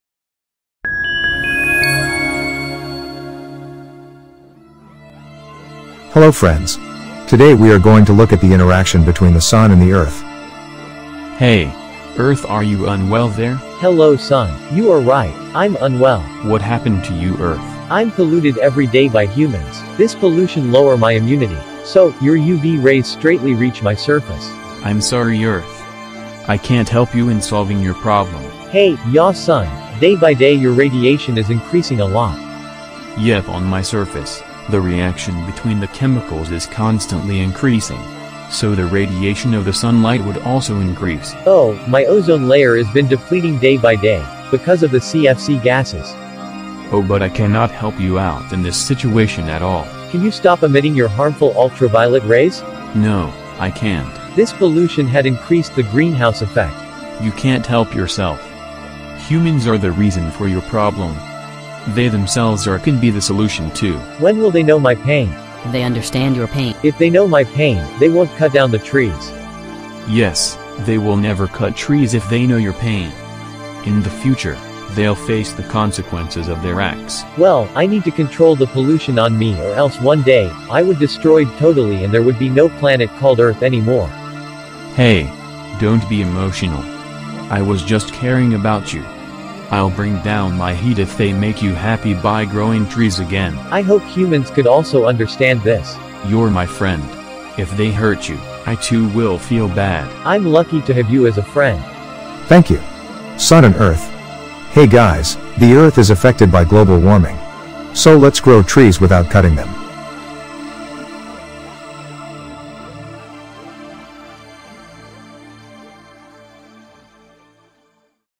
The Conversation Between The Sun & Earth
Interaction between The Sun and The Earth

Gspace : Hello friends !!!  Today we are Going to look at the

Sun : Hey!!! Earth are you unwell there?

Earth : Hello Sun. You're right. I'm unwell.

Sun : What Happened to you Earth?

Earth: I'm polluted every day by humans.
This pollution lower my immunity.So,your UV rays straightly  reach my surface.

Sun: I'm sorry Earth. I can't help you in solving your problem.

Earth : Hey !!! yah Sun day-by-day your radiation is increasing a lot.

Sun : yep on my surface the reaction between the chemicals is constantly  increasing so the radiation of the Sun light would also increase.

Earth : Oh, my ozone layer is been depleting day-by-day because of the CFC gases.

Sun : oh but I cannot help you out in this situation at all.
Earth: Can you stop emitting your harmful ultraviolet rays?

Sun: No, I can't.

Earth: This pollution had increased the greenhouse effect.

Sun: You can't help yourself. Humans are the reason for your problem. they themselves are can be the solution too.Earth: When will they know my pain?

Sun: They understand your pain.

Earth:If they know my pain, they won't cut down the trees.

Sun: Yes, they'll never cut trees if they know your pain. In the future, they'll face the consequences of their acts.

Earth: Well ,I need to control the pollution on me or else one day I would destroyed totally and there would be no planet called Earth any more.

Sun : Hey! Don't be emotional. I was just caring about you. I'll bring down my heat if they make you happy by growing trees again.

Earth: I'm lucky to have you as a friend.

Sun: You're my friend. If they hurt you, I too will feel bad.

Earth: I hope humans could also understand this.

Gspace : Thank You !!! Sun And Earth.

Gspace : Hey Guys, The earth is affected by global warming. So let's grow trees without cutting them.